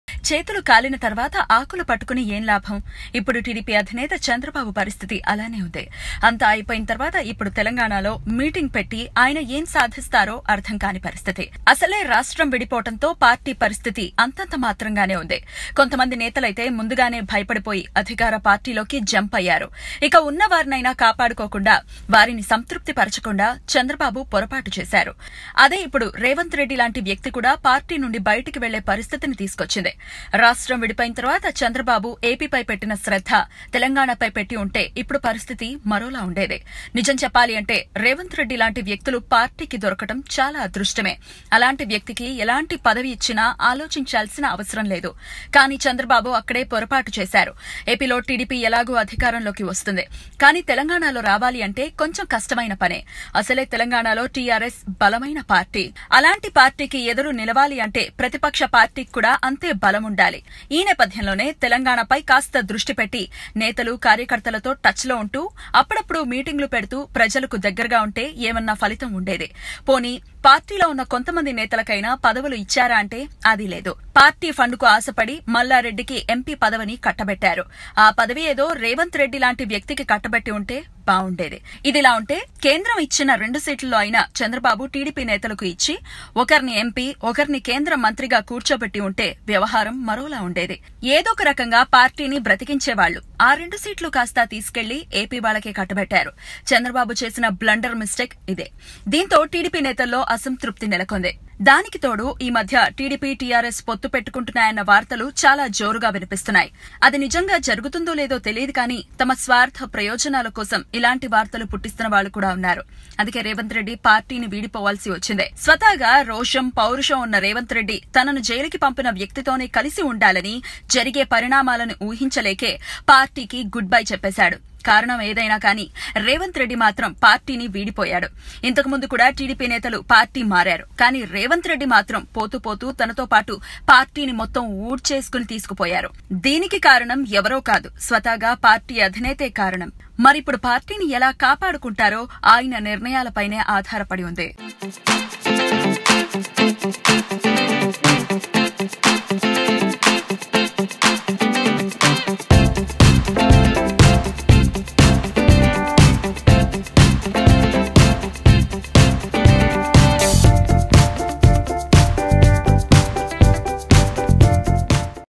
That Silly Mistake Committed By Chandrababu | Telugu Latest News | NewsOne
Chandrababu Naidu has completely neglected Telangana. He will be conducting a meeting here. But he's too late. Watch this video to know what is the mistake committed by Chandrababu Naidu...